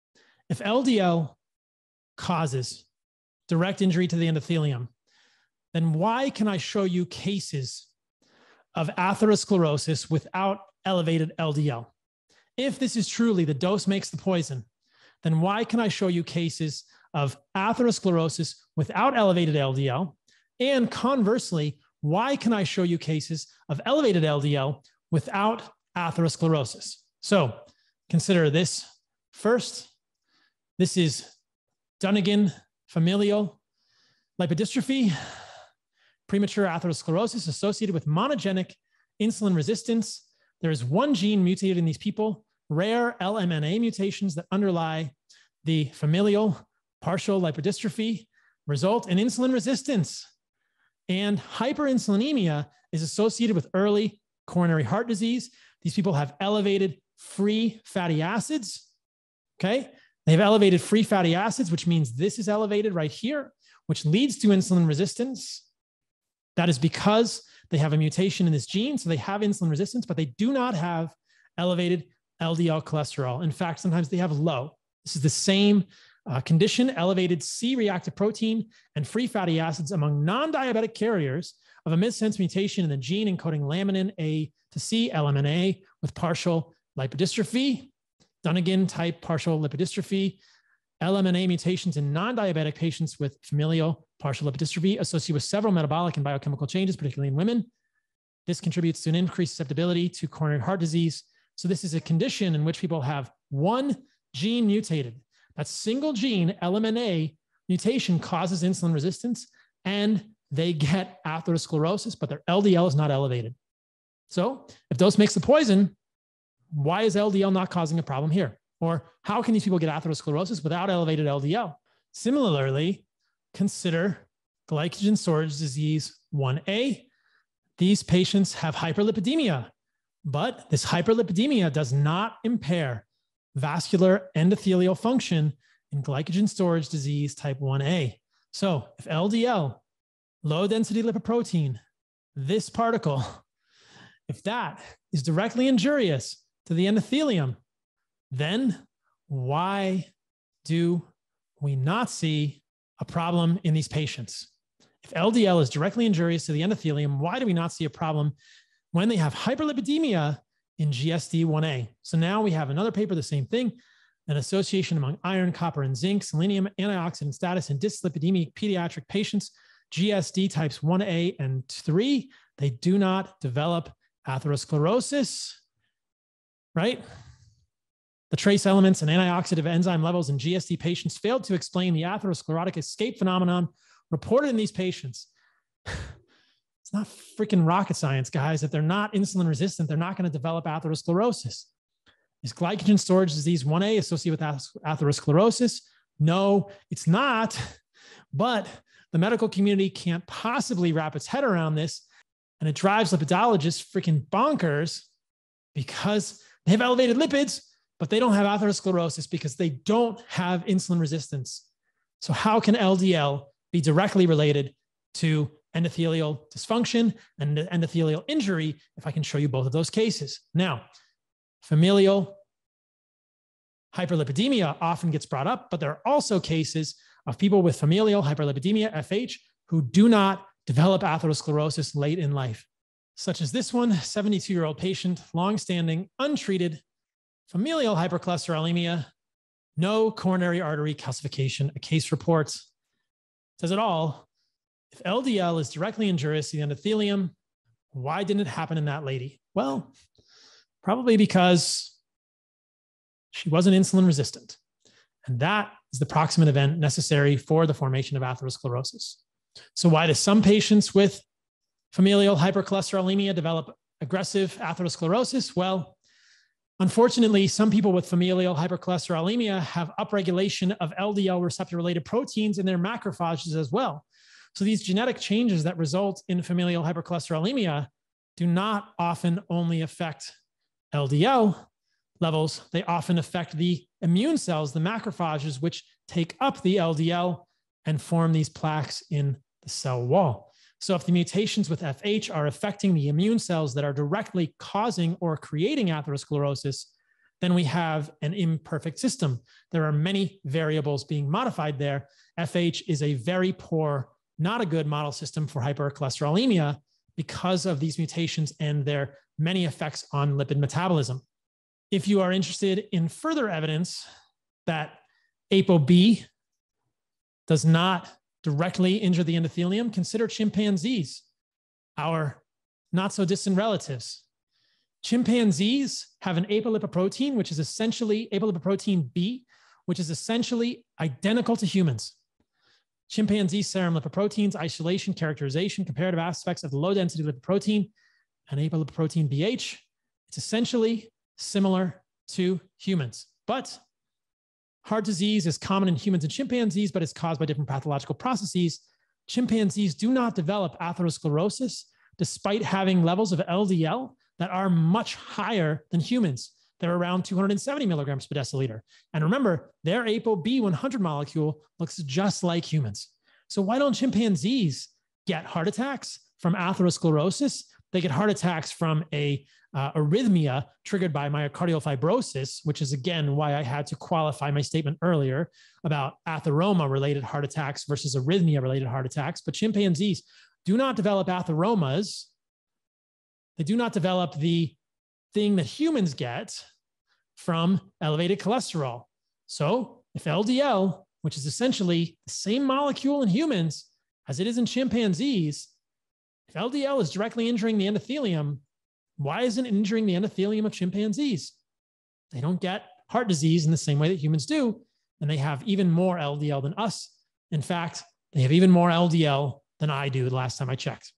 Elevated LDL does not cause atherosclerosis
Paul shares studies exhibiting cases of atherosclerosis without elevated LDL and cases of elevated LDL without atherosclerosis. He explains that insulin resistance rather than elevated LDL contributes to endothelial dysfunction.